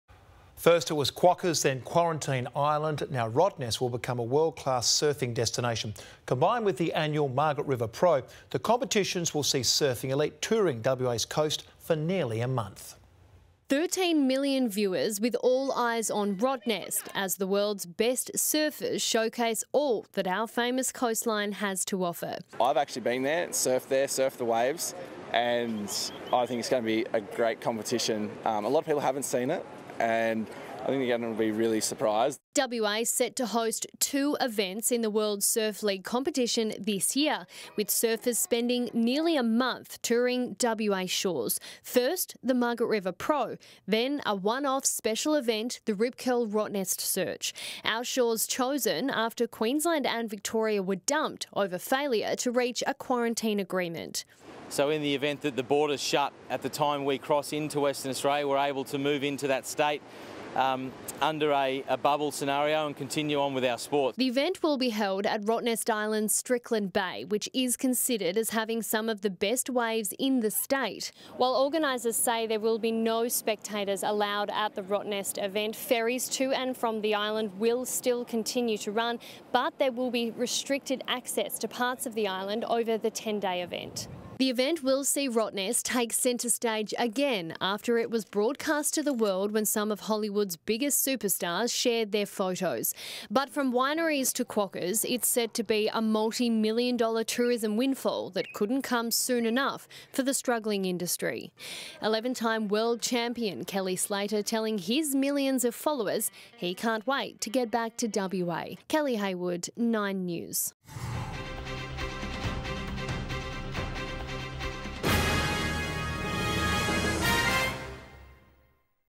Rotto becomes world surfing destination I 9News Perth
Rottnest Island will become a world surfing destination, combined with the annual Margaret River Prop.
9News | Nightly at 6.00pm